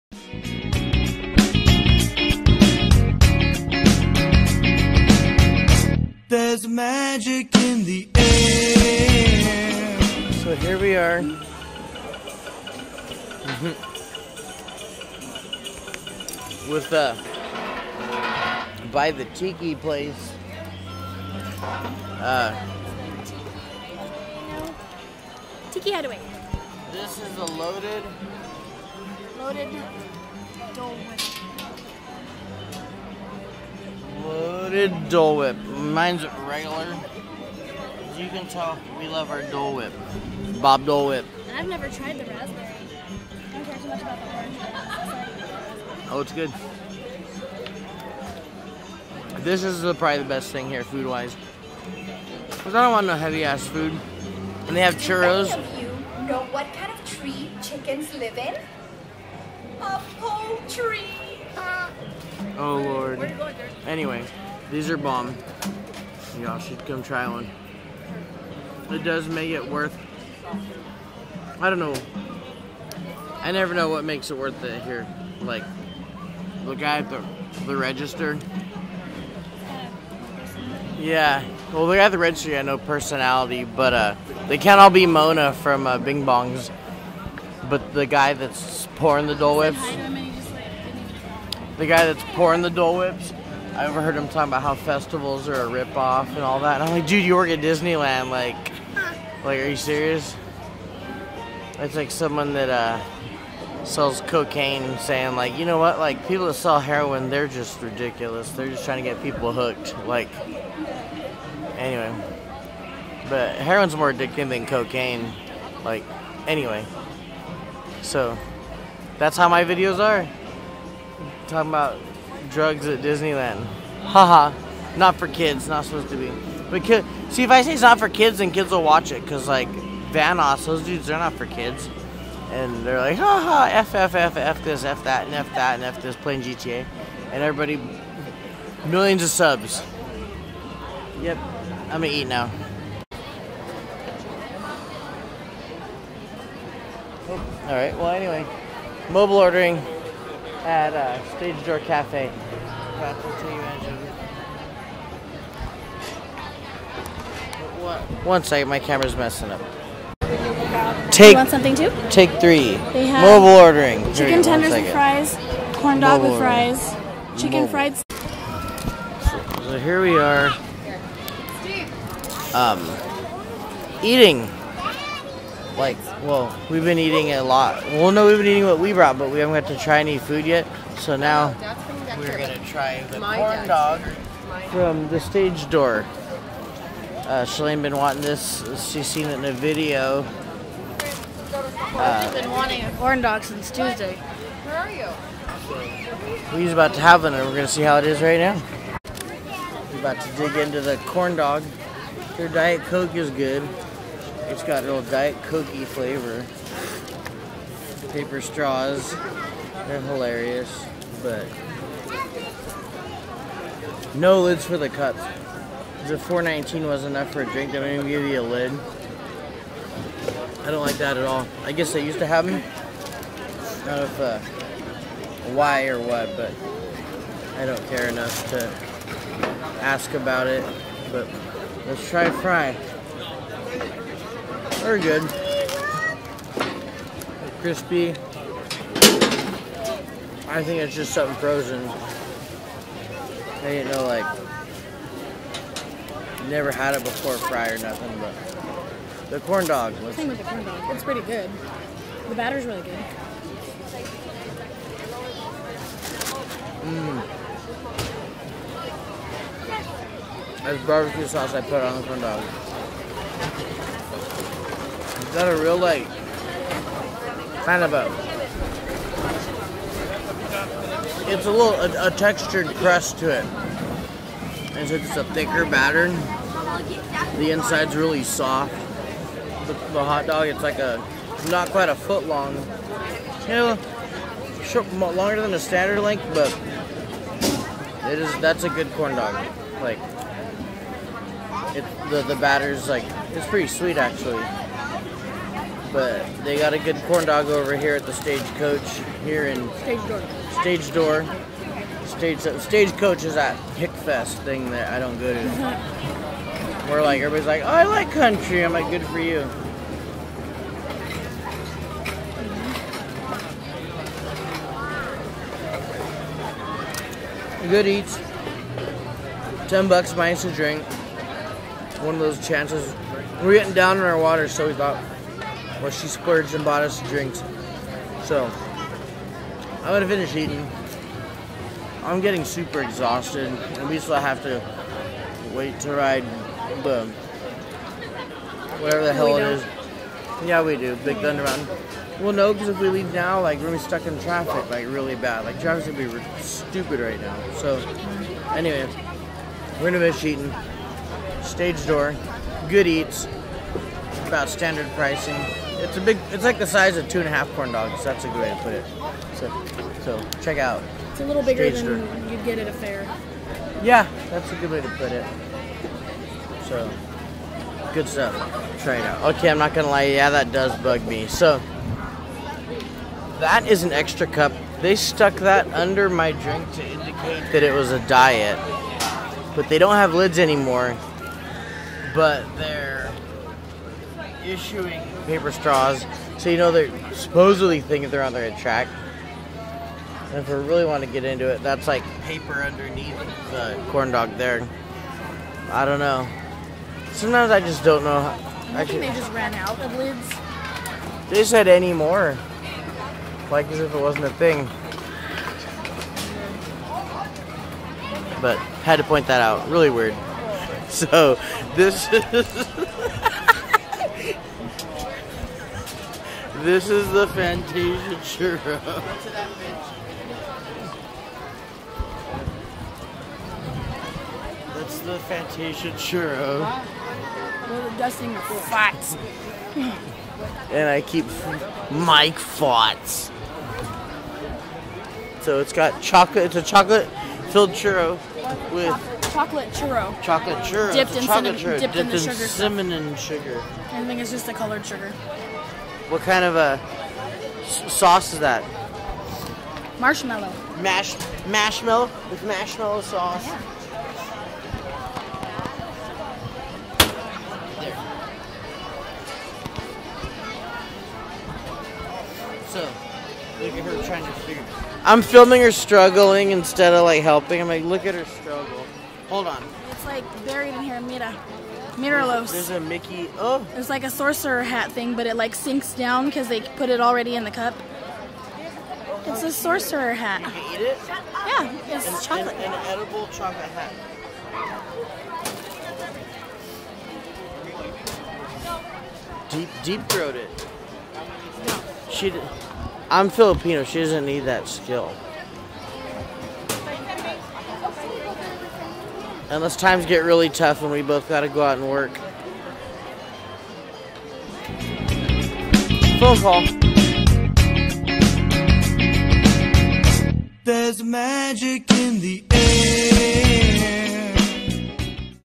HimGirl Travel Disneyland Poods
Cealane and I try out a few food items on day 3 of our Disneyland trip in late May of 2019